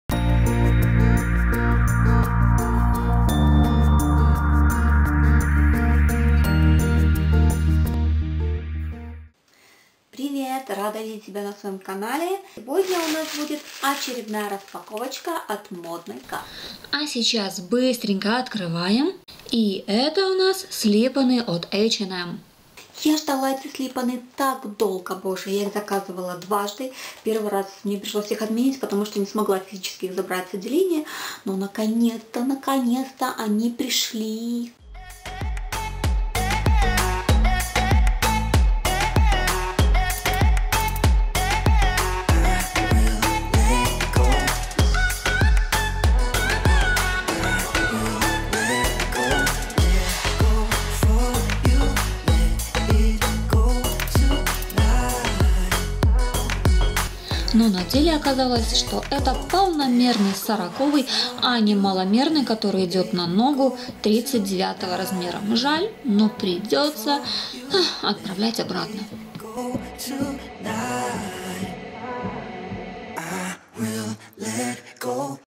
Обувь H&M. Распаковка #2 от ModnaKasta
Обувь H&M. Распаковка #2 от ModnaKasta это новое видео с распаковкой класснейших слипонов от H&M.

Снимала я очень быстро, поэтому видео Обувь H&M. Распаковка #2 получилось коротеньким. Основные моменты я показала, а все описание к данной обуви вы найдете ниже:

О товаре:
- Акция:  Обувь H&M
- название: Слипоны H&M
- Слипоны H&M
- Сезонность: Лето
- Состав: верх: текстиль/внутри: текстиль
- Цвет: Белый
- Страна производства: Китай
- Цена: (оригинальная 1950 грн), на сайте 449 грн
- Размерная метка: НЕ МАЛОМЕРЯТ!!! (как указано на сайте), полномерный размер на среднюю полноту или широкую стопу!